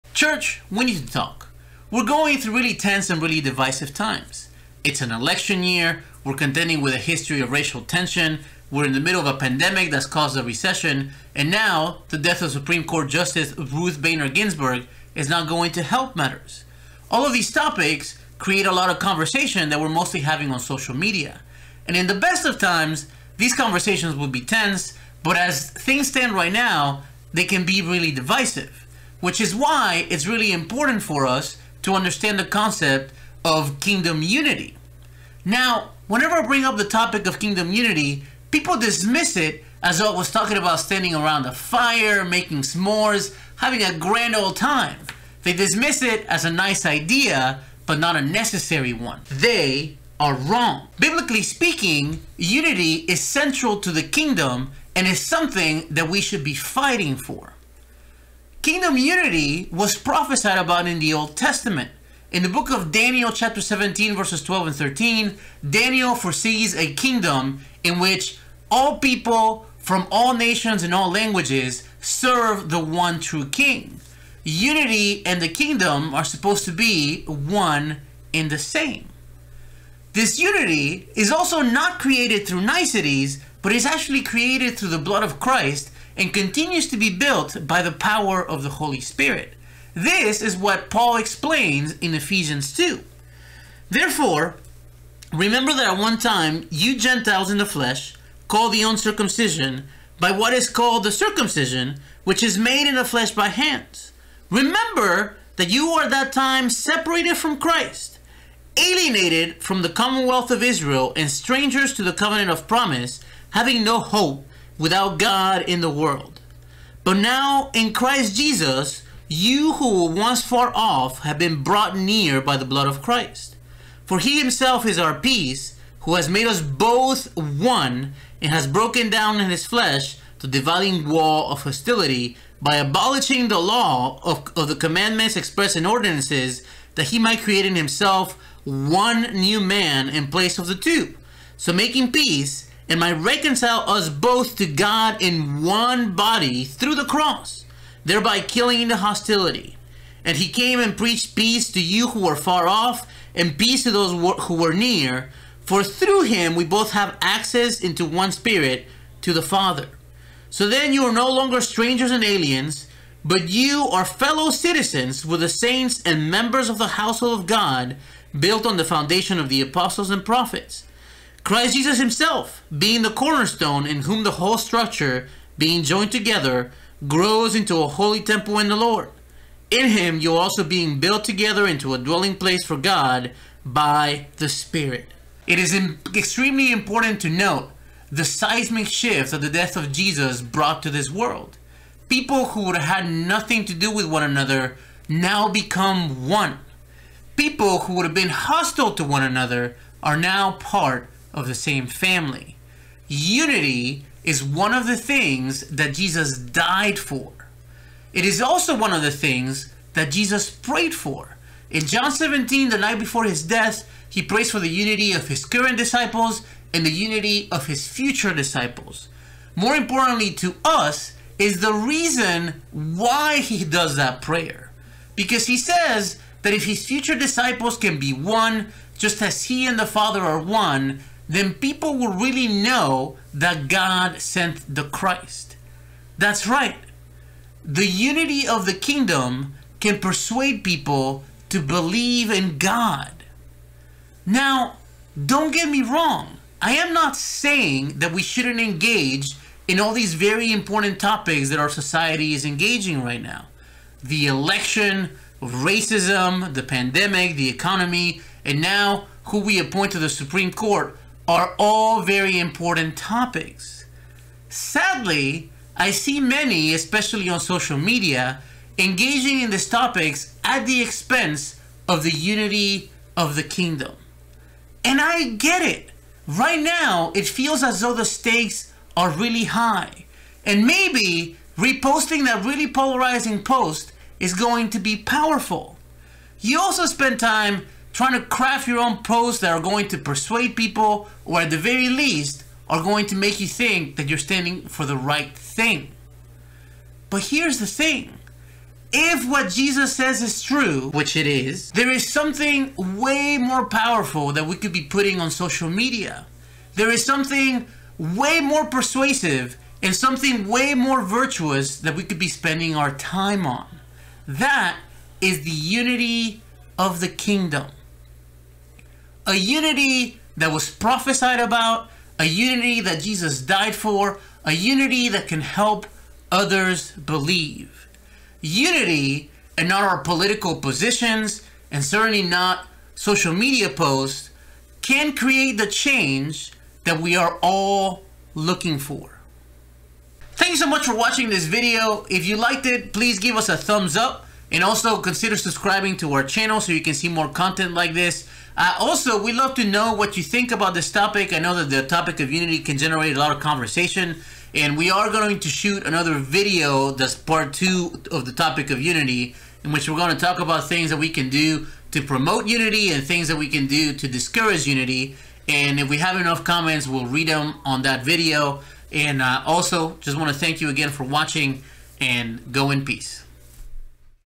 Unity in the Church Pt1
We are going through a time of great division. As a society, we are discussing very important topics right now. The election, racism, the COVID 19 pandemic, the economic aftermath of said pandemic and now, who we are going to appoint to the supreme court. All of these are important topics that in the best of times can create tension but as things stand right now, they create division. Which is why it is really important that Christians really understand the concept of unity.